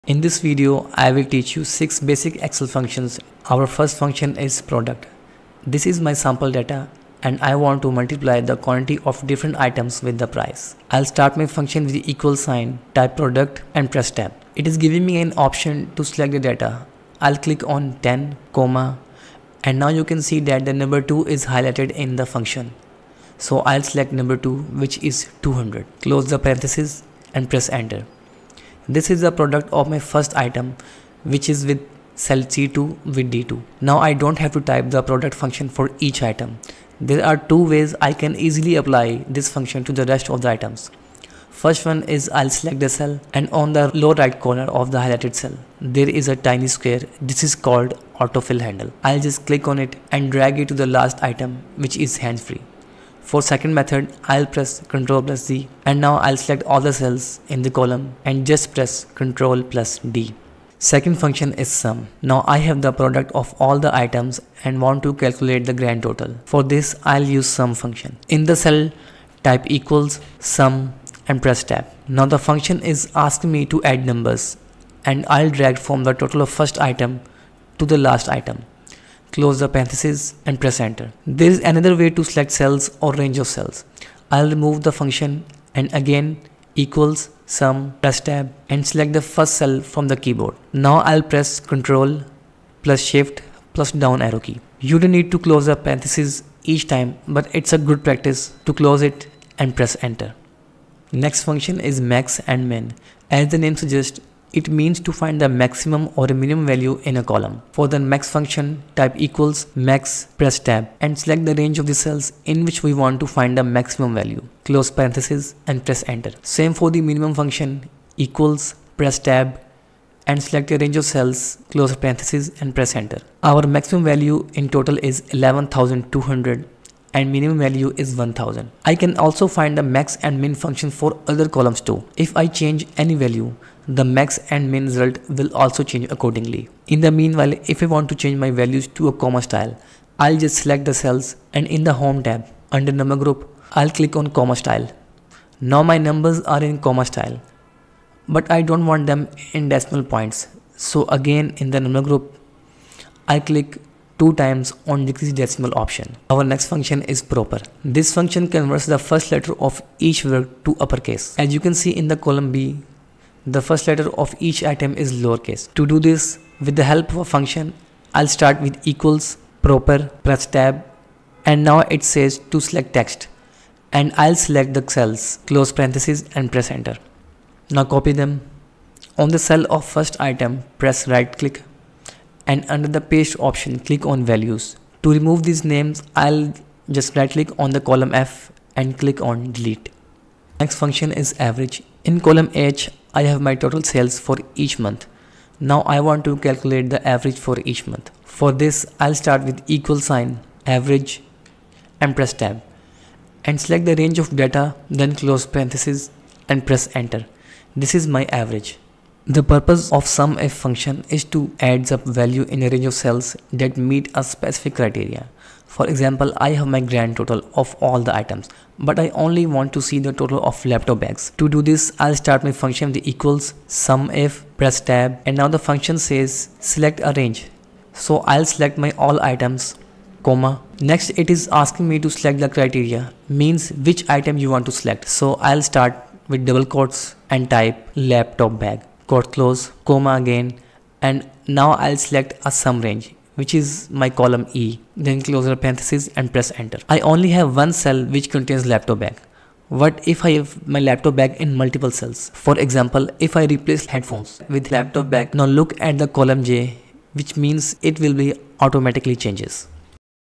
Top 6 Excel Functions to Learn
In this video you will learn 6 basic excel functions that are very helpful during excel. These functions are =PRODUCT, =SUM, =MAX, =MIN, =PROPER, =AVERAGE, and =SUMIF.

Basic excel functions
Excel functions for beginners
How to Excel functions
Important Excel functions
How to use Excel functions

Timestamps:
00:00 Product =PRODUCT
01:00 Sum =SUM
01:41 Max & Min =MAX & =MIN
02:36 Proper =PROPER
03:10 Average =AVERAGE
03:29 Sumif =SUMIF